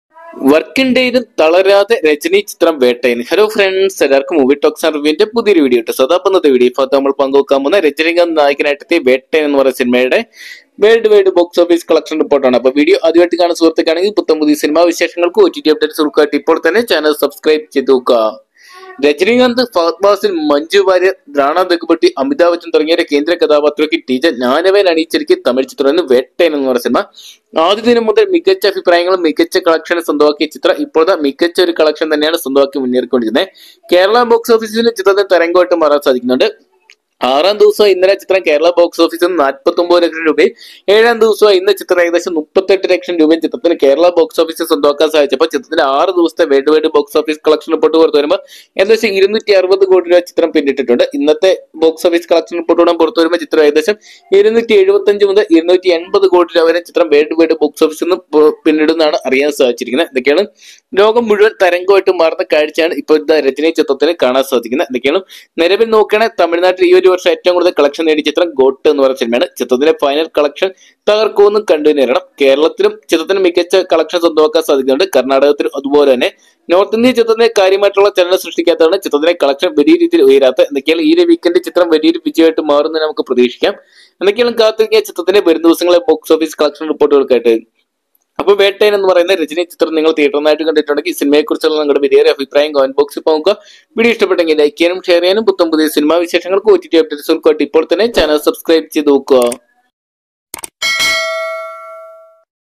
ആദ്യ വാരം G.O.A.T നെ തകർത്ത് രജനി ചിത്രം വേട്ടയ്യൻ🔥 | Vettaiyan Kerala 7 Day's Worldwide Collection
ബുധനാഴ്ച കേരളത്തെ വിറപ്പിച്ച് രജനി ചിത്രം വേട്ടയ്യൻ🔥 | Vettaiyan Kerala 7th DAY Kerala Collection
Vettaiyan Kerala 3rd DAY Collection | 3 Day's WW Collection